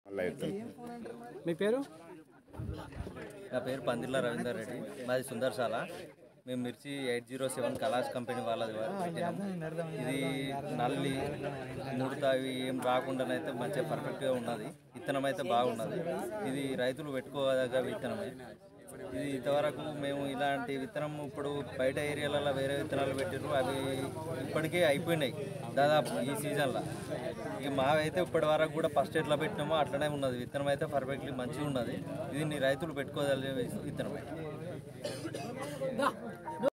కలష్ సీడ్స్ 807 మిర్చిరకము విరగా కాసే రకము,ఎకరాకు35క్వింటాళ్ల పైన దిగుబడి,ఆసక్తిగా చూస్తున్న రైతులు
Rathnakar reddy@mettu